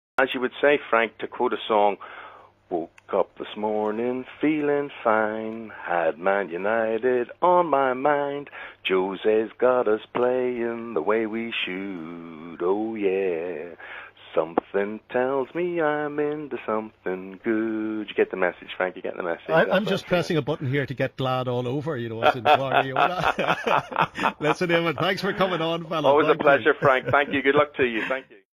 Eamonn Holmes sings to Frank Mitchell
Which is more likely... Eamonn Holmes making it into the Charts or Manchester United making it into the top 4?